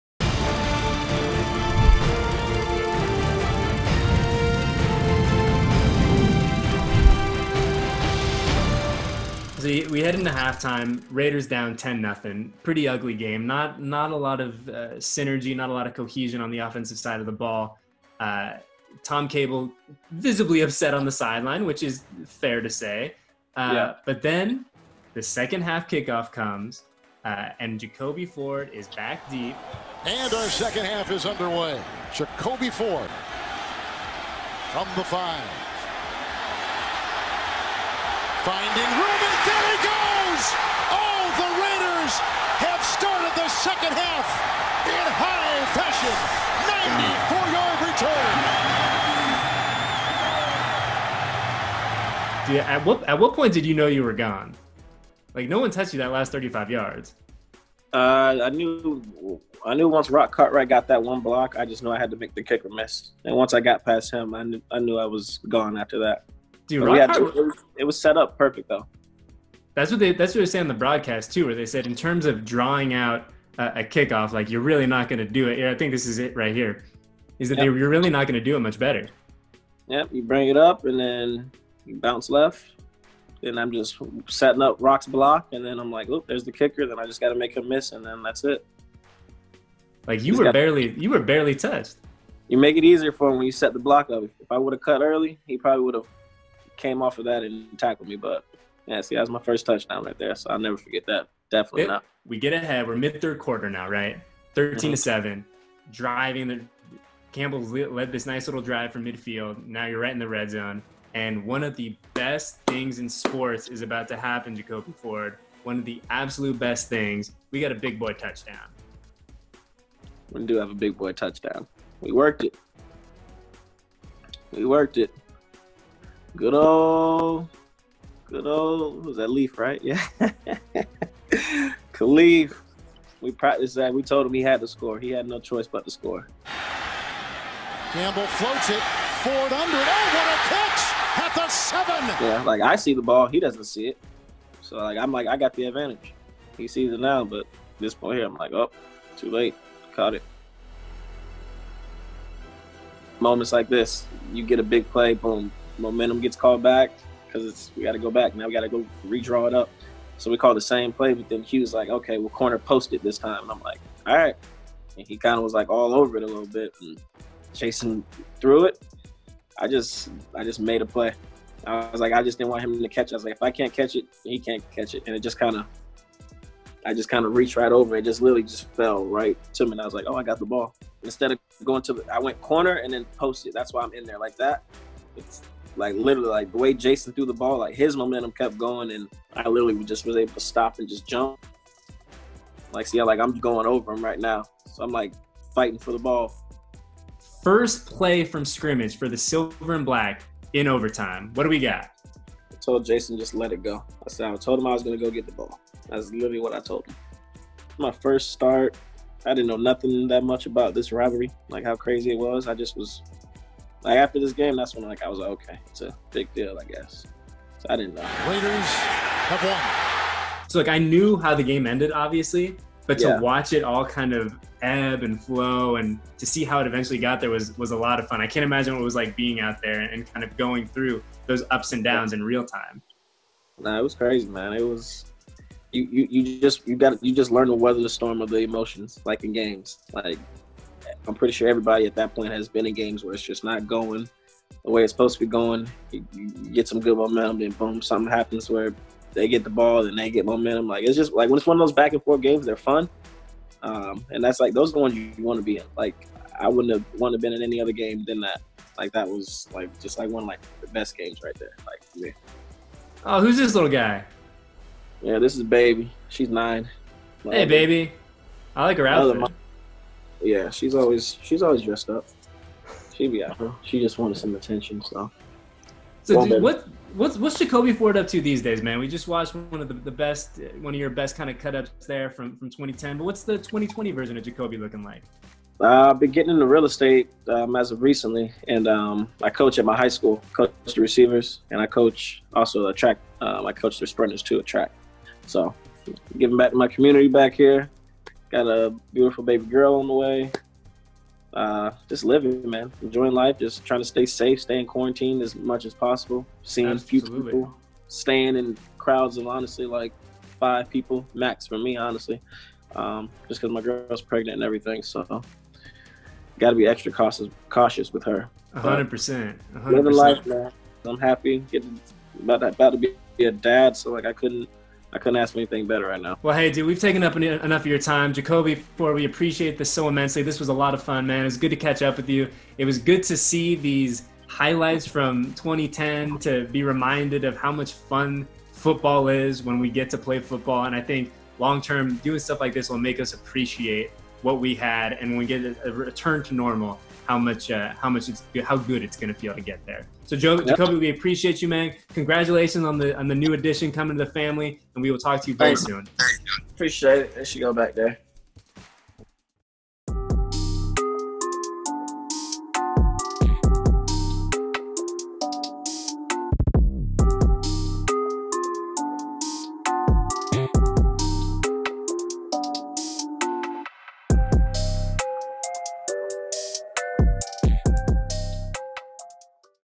Jacoby Ford Relives His First NFL Touchdown | Raiders Playback
Raiders.com's Eddie Paskal sits down with former wide receiver Jacoby Ford to discuss his first NFL touchdown and his memorable game against the Kansas City Chiefs in 2010.